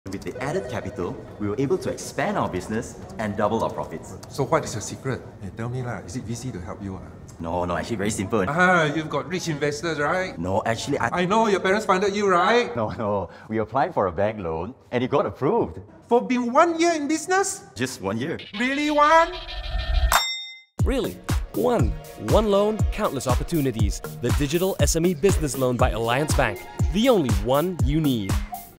Really Just 1 Year in Business Can Apply?!
Fast track your business growth and get fast access to funds with Alliance Digital SME. Businesses with minimum 1 year in operations can apply!

*T&Cs apply